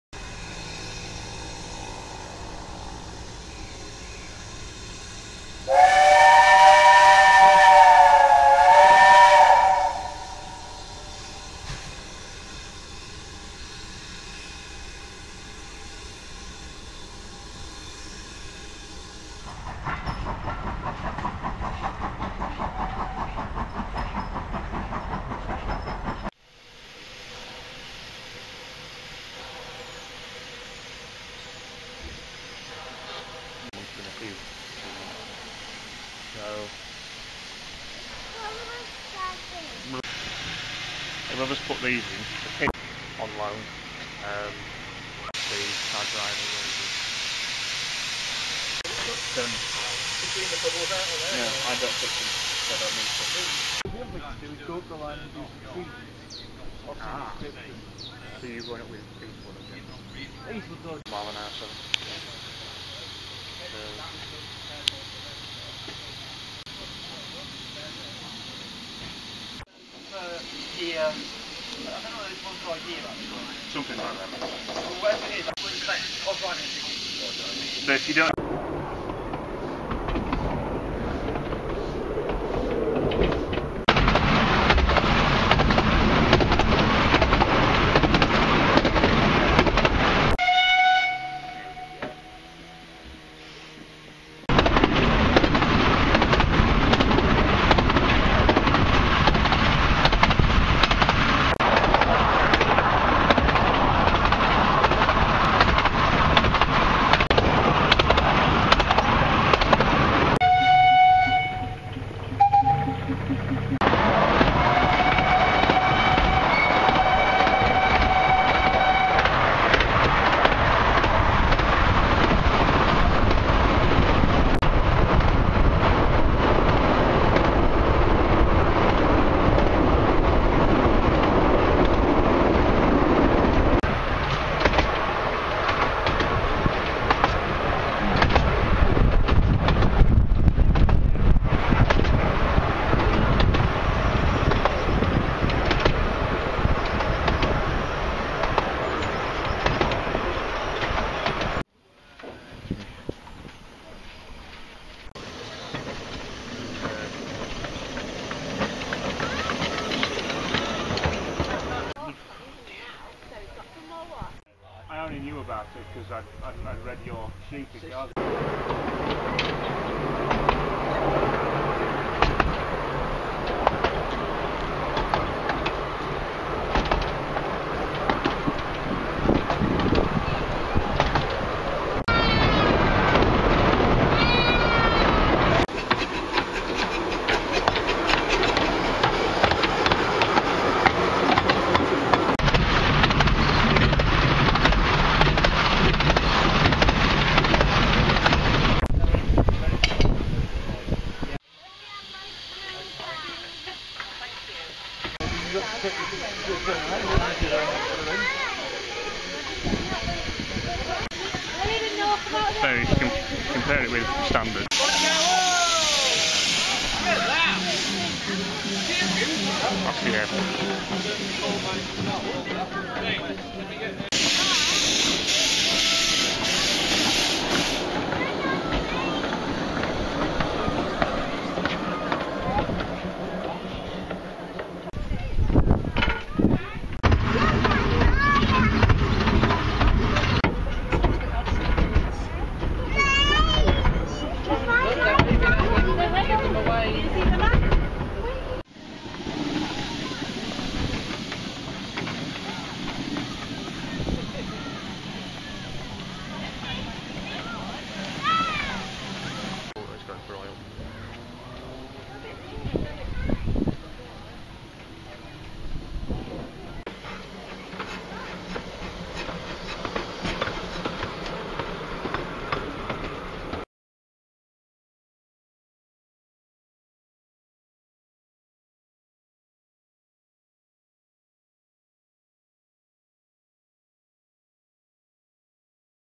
15 inch Gauge Train trip at Kirklees. Feb 2020
Love a little trip on a 15 inch gauge railway, always something special! Who didn't ride one of these when they were younger!